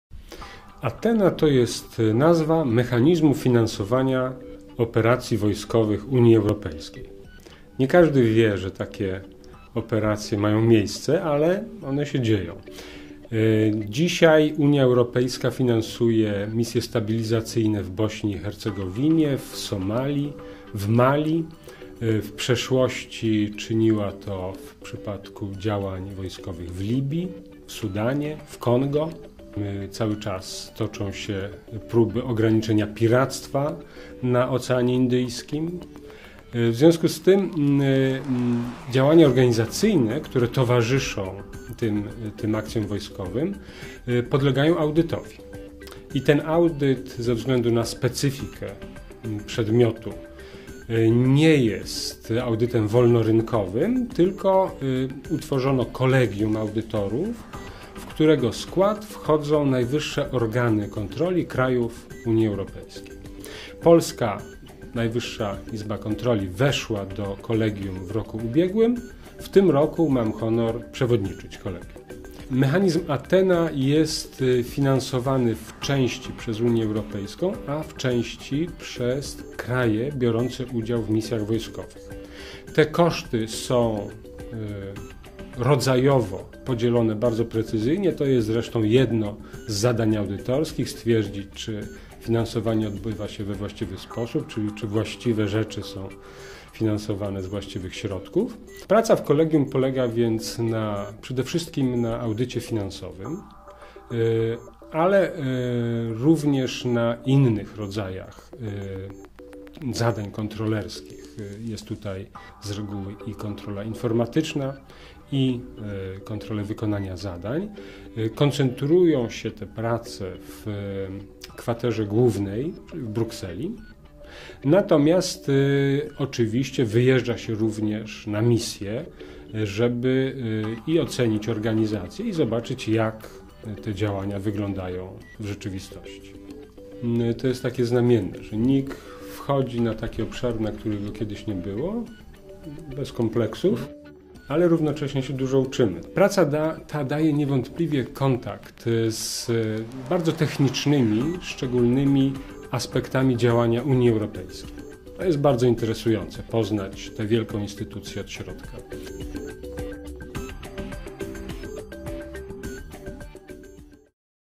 Projekt ATHENA UE
NIK uczestniczy w audycie finansowania misji wojskowych UE. O pracy polskich kontrolerów opowiada Paweł Banaś, przewodniczący Kolegium Audytorskiego ATHENA UE